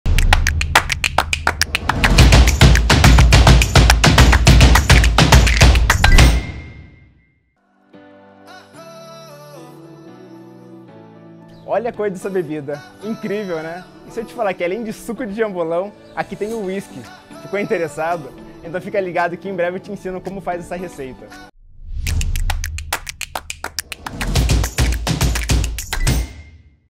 Drink Jambo Royale - Santa Bárbara Resort Residence
Para a 2ª Jambolão Fest, que é realizada no SBRR, o chef Iran Rodrigues criou novas receitas para incluir o jambolão no seu cardápio.